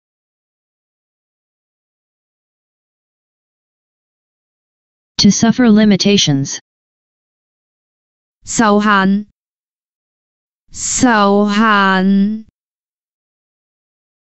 "TO SUFFER LIMITATIONS" in Cantonese (受限) - Flashcard
[sau6 haan6] "TO SUFFER LIMITATIONS" in Cantonese (受限) - Flashcard. Additional information about the word: to suffer limitations.